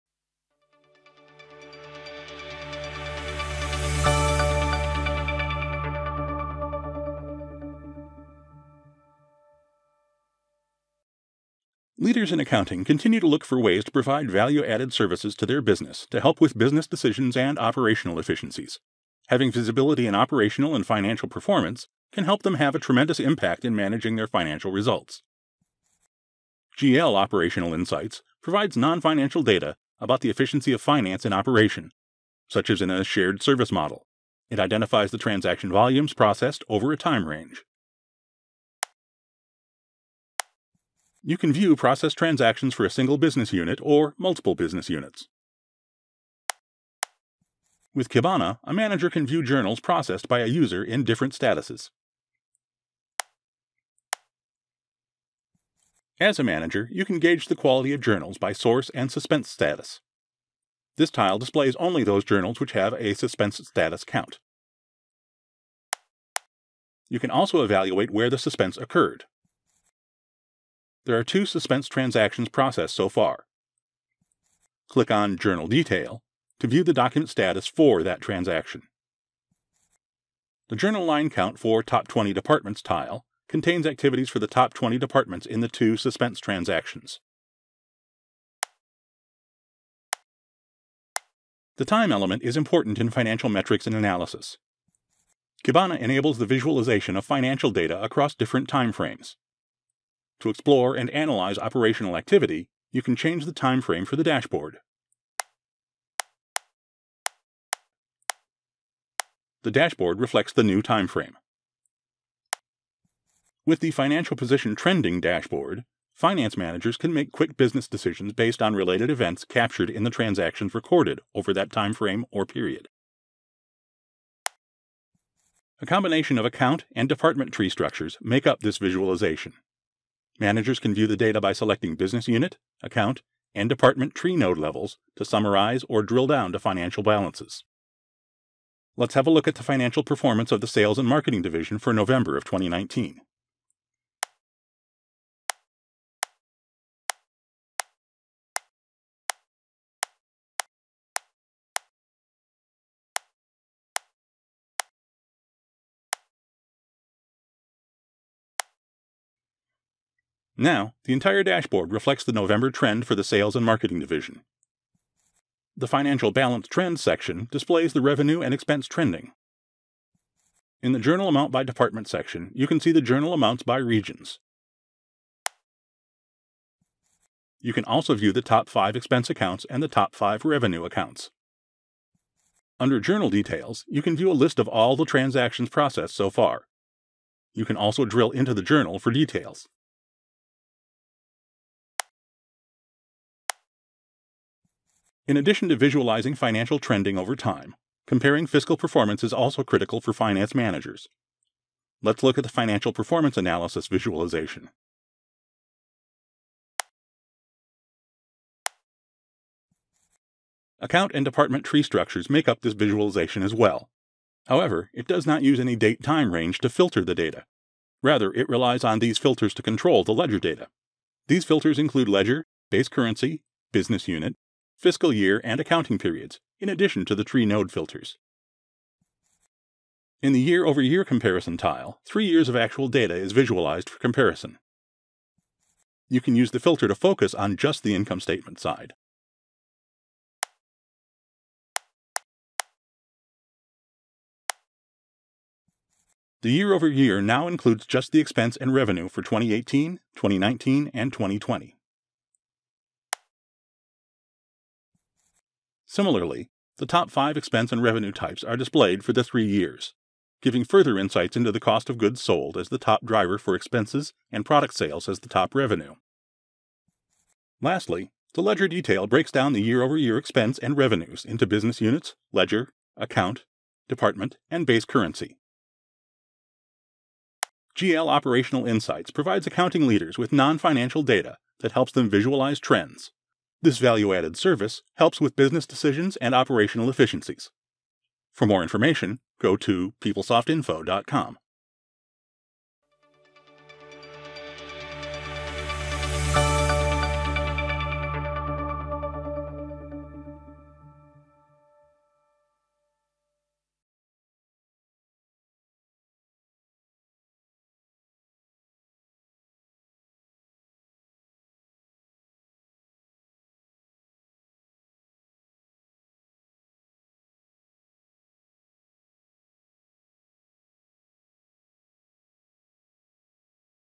PeopleSoft GL Kibana - Insight, Trending, and Analysis
Leaders in Accounting continue to look for ways to provide value-added services to their business to help with business decisions and operational efficiencies. Having visibility in operational and financial performance can help them have a tremendous impact in managing their financial results. GL Operational Insights provides non-financial data about the efficiency of Finance in operation, such as in a shared service model. It identifies the transaction volumes processed over a time range.